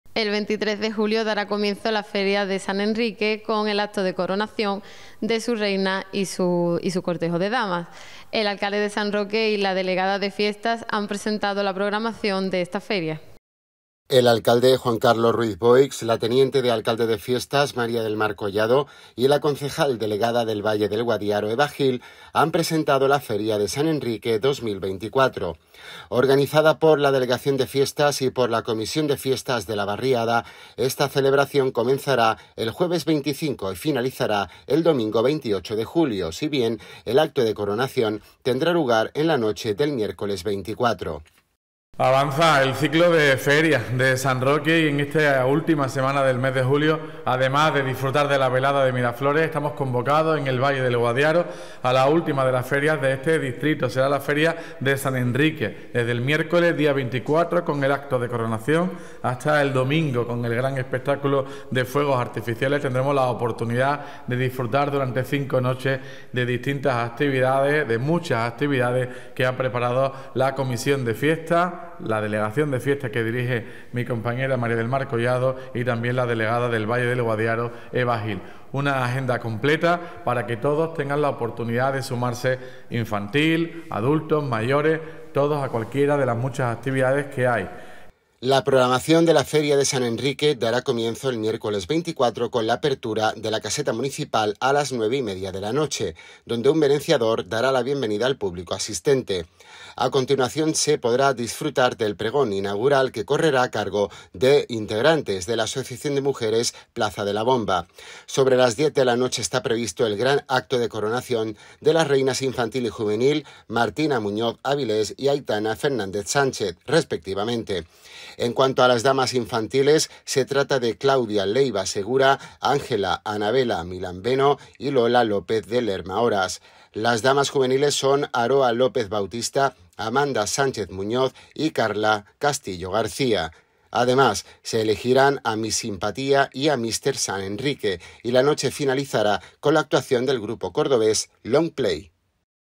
NOTICIA PRESENTACIÓN FERIA DE SAN ENRIQUE 21 JULIO 2024
El alcalde, Juan Carlos Ruiz Boix, la teniente de alcalde de Fiestas, María del Mar Collado, y la concejal delegada del Valle del Guadiaro, Eva Gil, han presentado la Feria de San Enrique 2024. Organizada por la Delegación de Fiestas y por la comisión de fiestas de la barriada, esta celebración comenzará el jueves 25 y finalizará el domingo 28 de julio, si bien el Acto de Coronación tendrá lugar en la noche del miércoles 24.